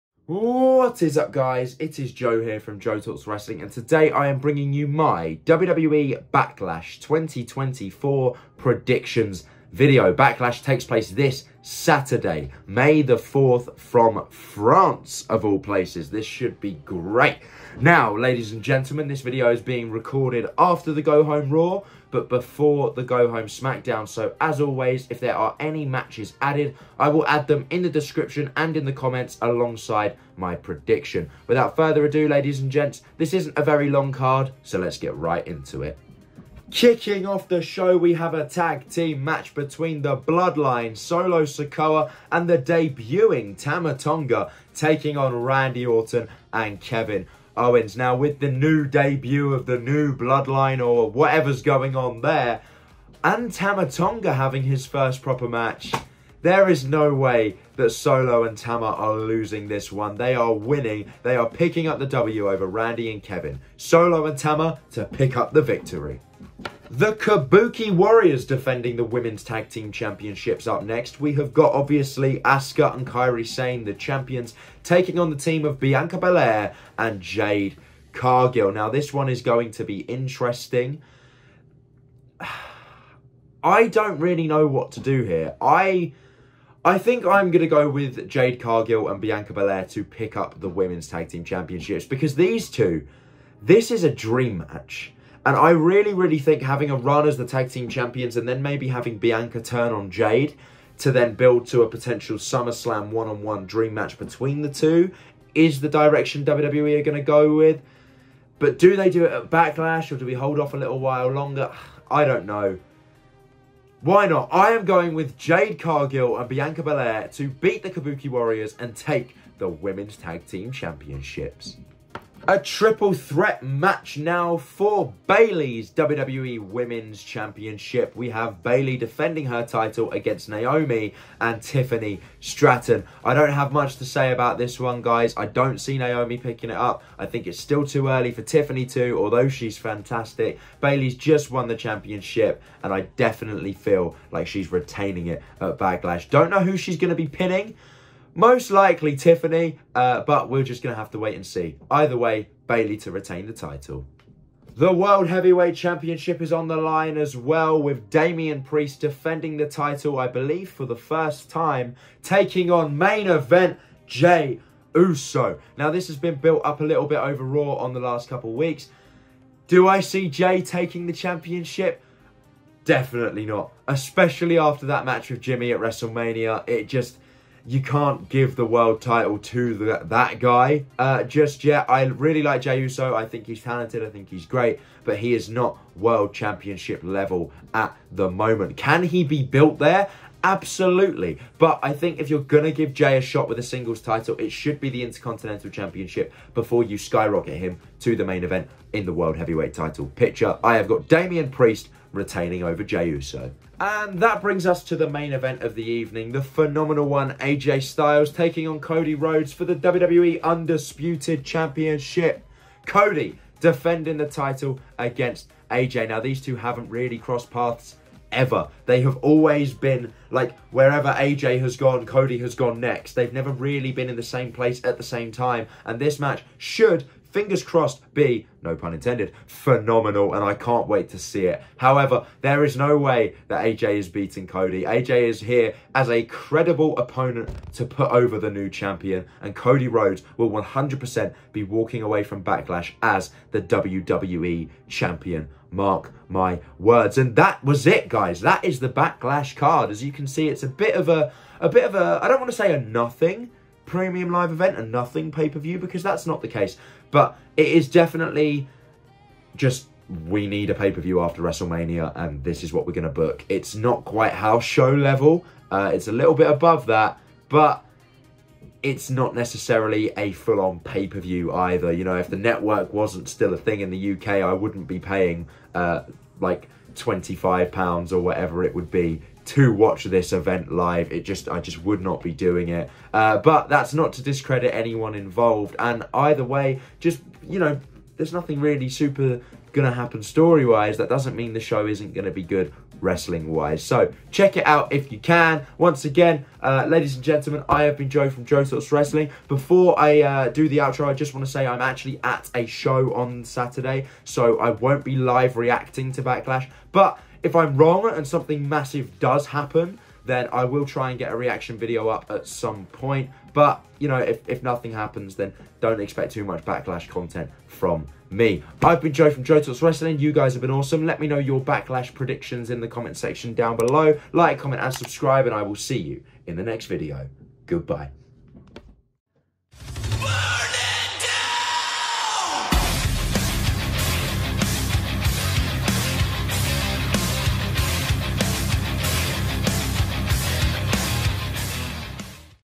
WWE BACKLASH 2024 PREDICTIONS!! - JoeTalksWrestling
Hey Everyone! In this video I give my predictions for this Saturday's WWE Premium Live Event (PPV) The 2024 Backlash event! This years Backlash  takes place at the LDLC Arena, located in the Decines-Charpieu area of Lyon, France.

There are 5 matches on the card as of right now but if any more matches get added after this video's release, I will add them in this description/comments along with my prediction.

The Backlash 2024 Match Card:
The Bloodline (Solo Sikoa & Tama Tonga) Vs Randy Orton & Kevin Owens
The Kabuki Warriors (Asuka and Kairi Sane) (c) Vs Bianca Belair & Jade Cargill - WWE Women's Tag Team Championship

Added Matches/Predictions:

What are your predictions for this years Backlash show?